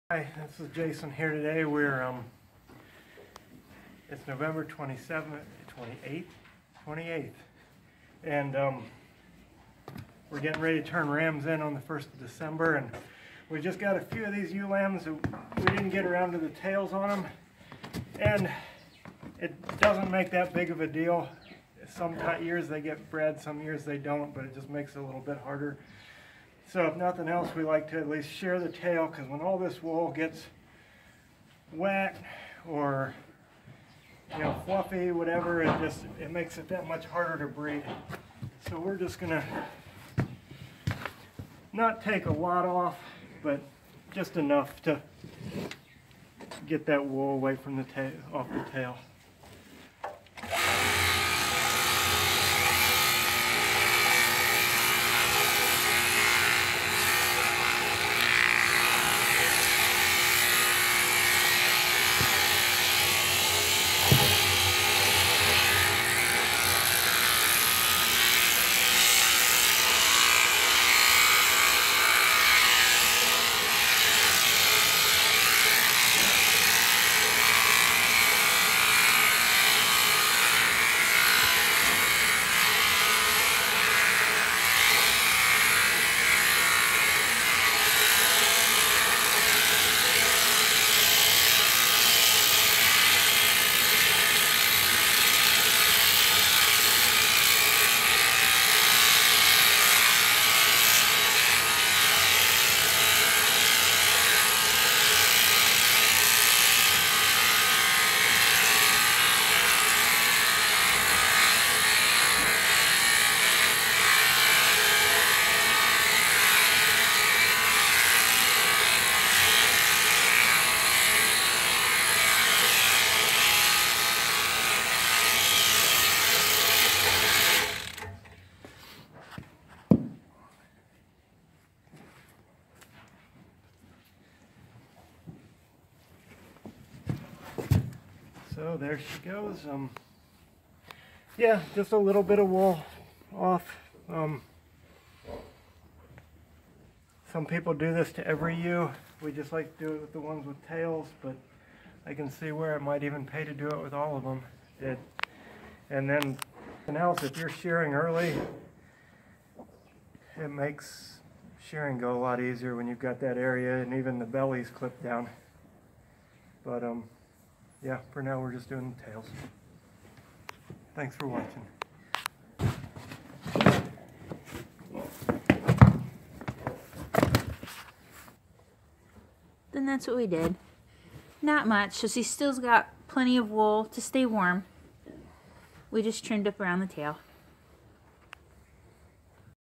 Crutching Ewe Lambs
Here's a quick and easy demo of crutching ewe lambs. Crutching would work for any age ewe, we just happen to have a ewe lamb to show.